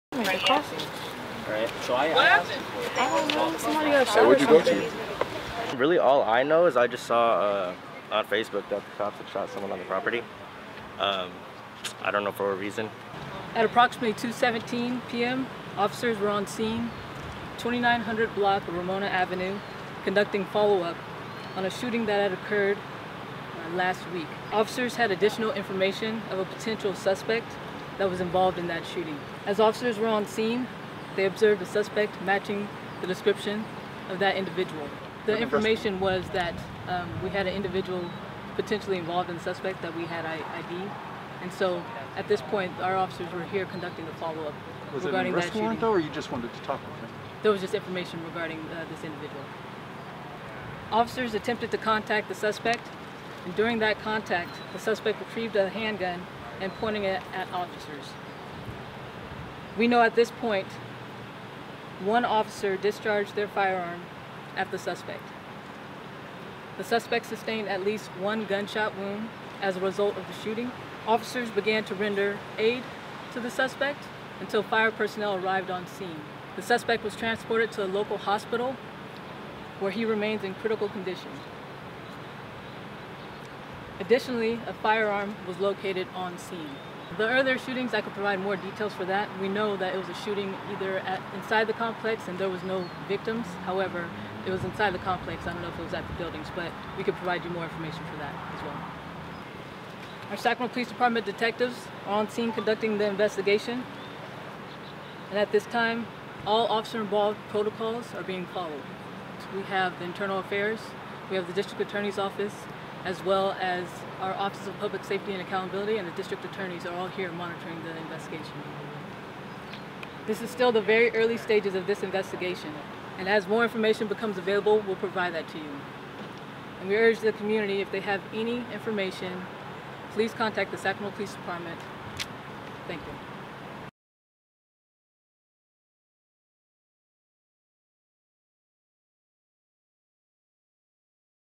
Police spokesperson describes officer-involved shooting near Sacramento State
An officer-involved shooting reported outside The Crossings apartment complex near Sacramento State on Tuesday, July 21, 2020. One person was taken to the hospital, according to reports, and no officers were injured.

Video by: Alie Skowronski